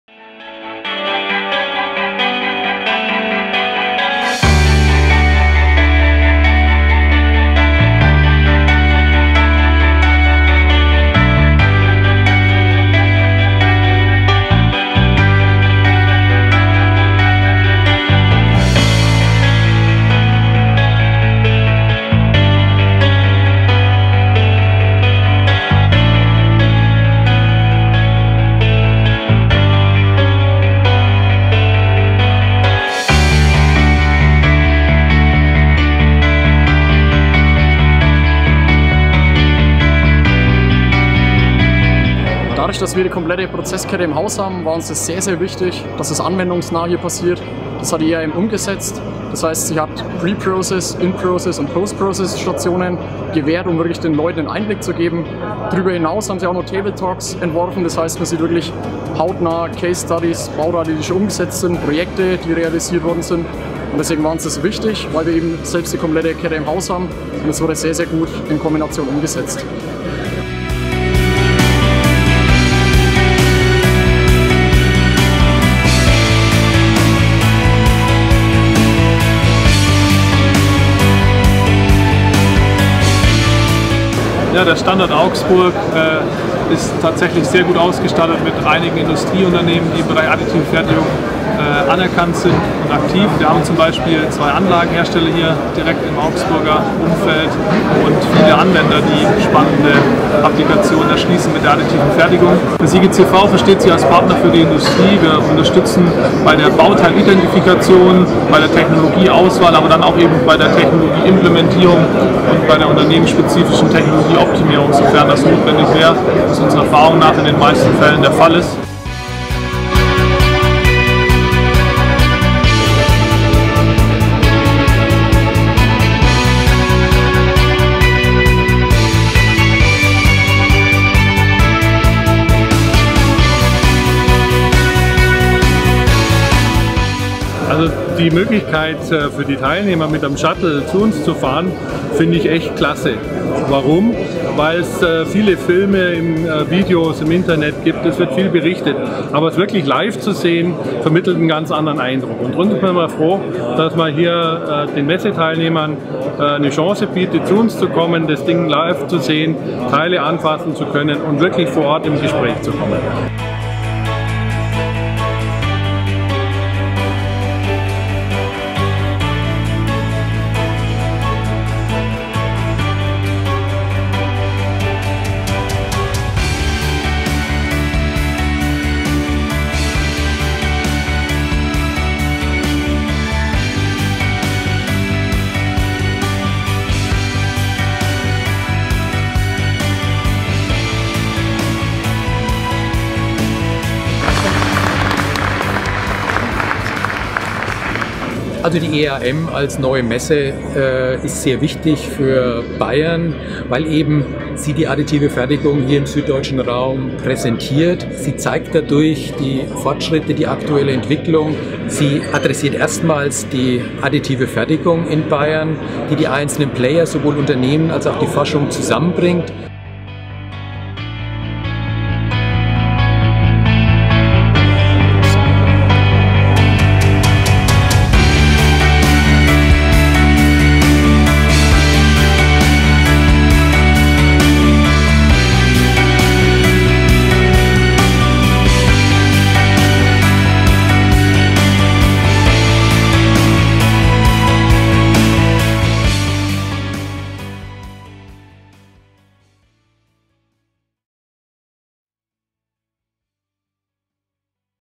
Film der EAM Premiere 2018
Film ab!
Verschaffen Sie sich einen Überblick von der äußerst erfolgreichen Messepremiere der EXPERIENCE ADDITIVE MANUFACTURING!
Stimmen Sie sich mit den filmreifen Eindrücken und Impressionen bereits jetzt auf 2019 ein!